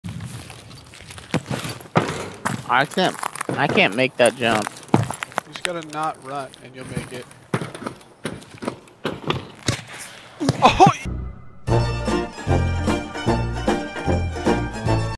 Showing a newer player how to jump Porta-potty
The customs shortcut jump-over just needs regular walking speed to jump onto the porta-potties. Unlucky timing of the guy at the end coming out from around the corner. R.I.P. Juice. o7